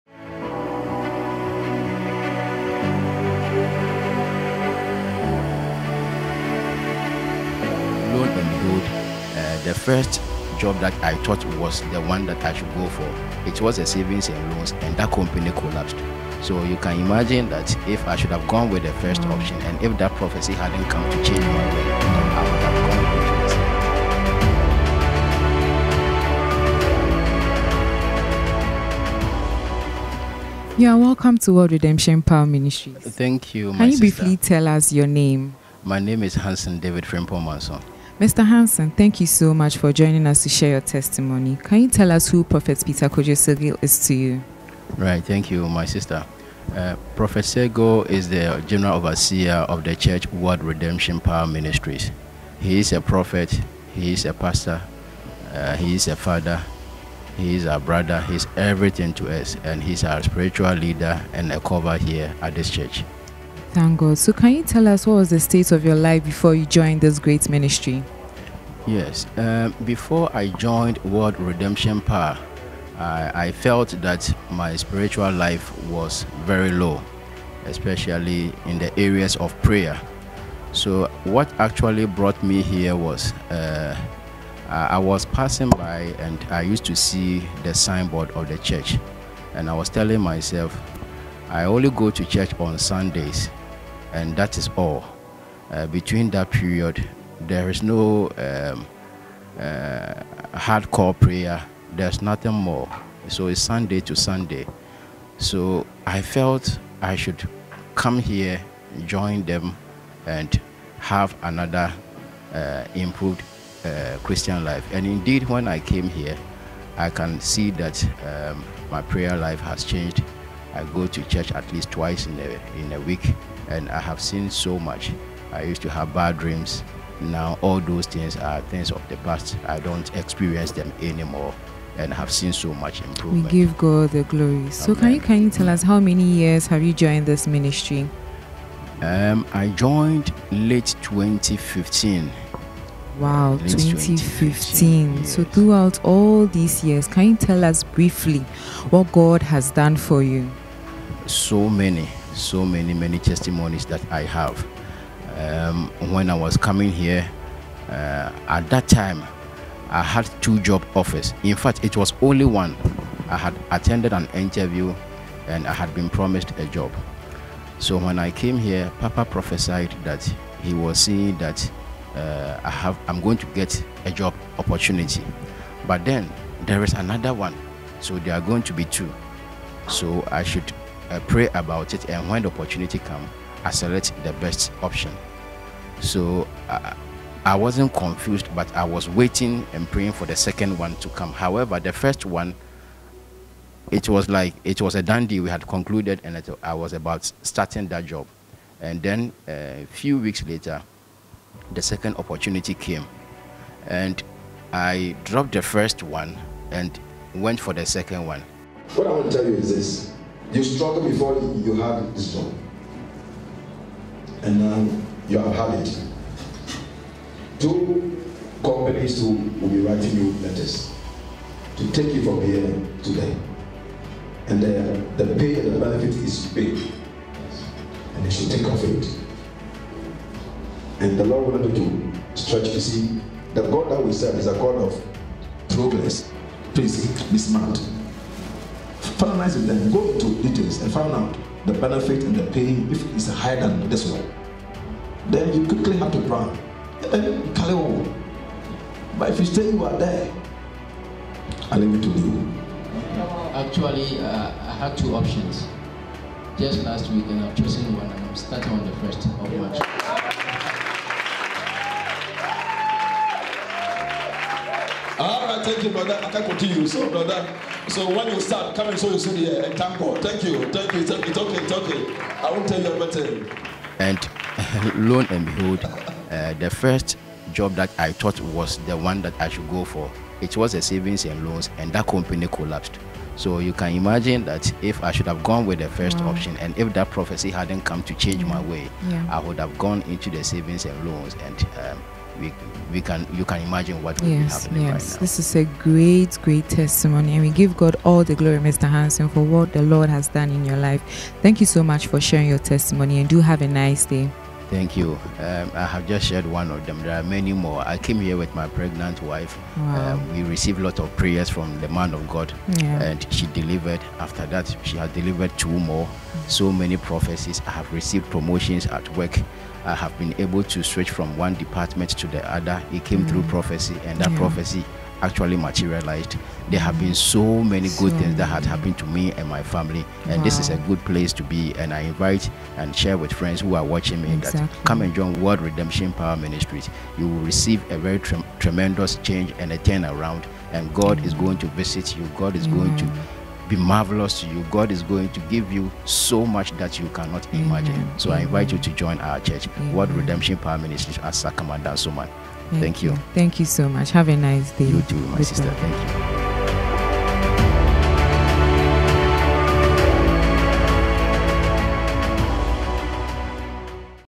Unbelievably Finding the Perfect Job | World Redemption Power Ministries Ghana
My Miraculous Journey to Unbelievably Finding the Perfect Job [Life-Changing Testimonies]

You can also fellowship with us in person at World Redemption Power Ministries auditorium Accra-Ghana (South-Odorkor Adj. ST. Anthony School)